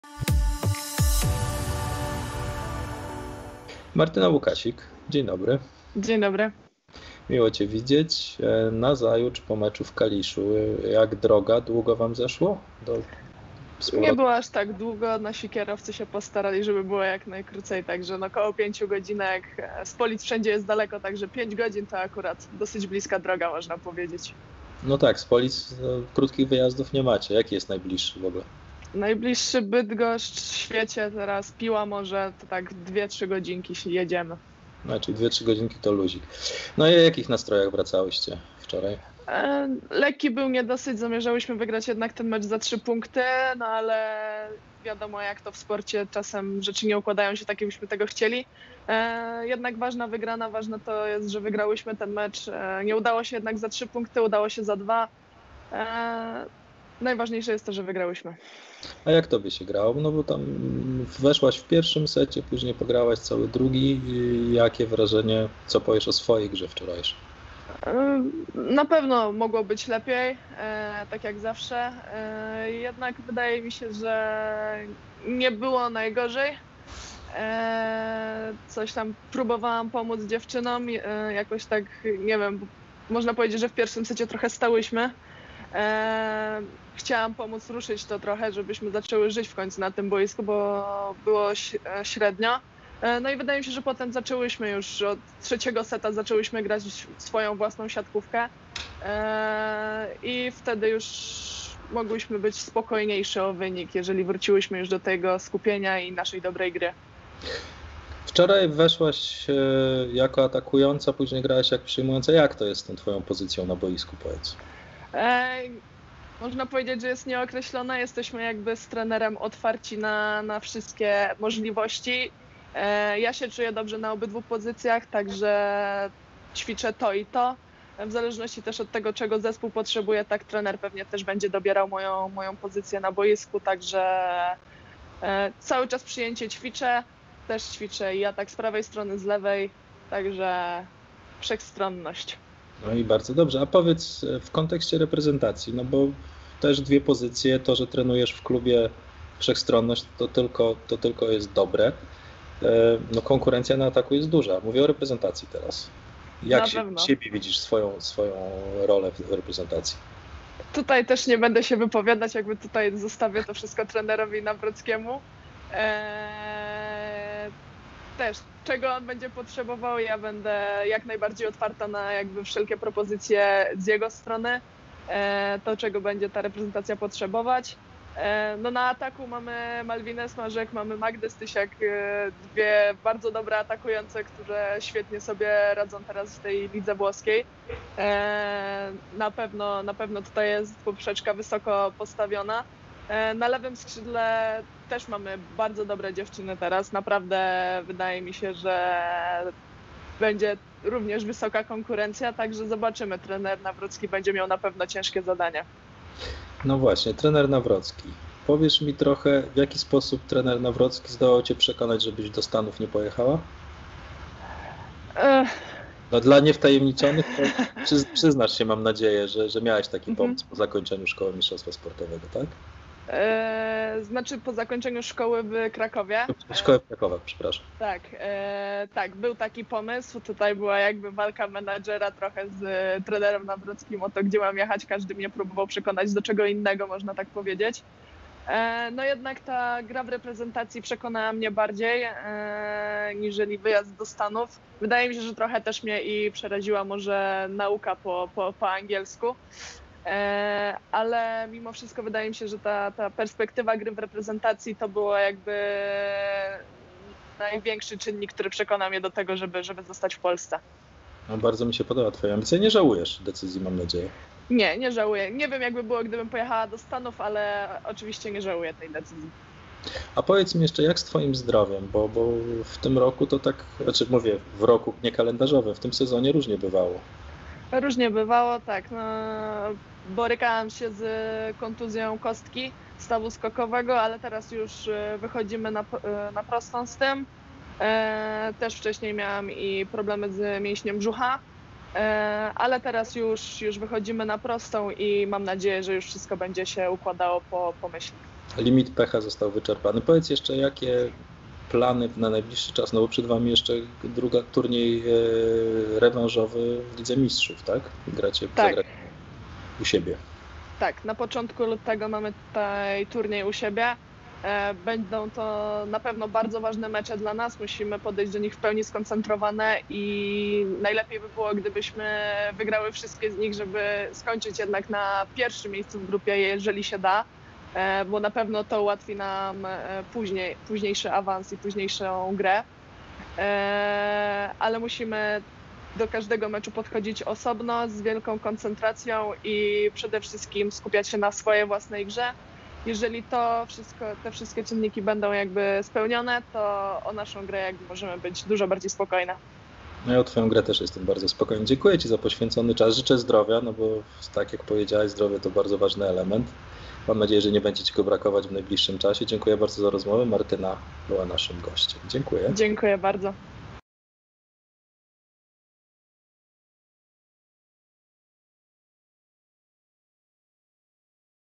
SiatkarskaPolska - Martyna Łukasik
Dwudziestodwuletnia siatkarka Grupy Azoty Chemika Police i reprezentacji Polski grająca na pozycji. , no właśnie przyjmującej czy atakującej? O rozterkach dotyczących nie tylko siatkarskiego życia w rozmowie z Martyną Łukasik.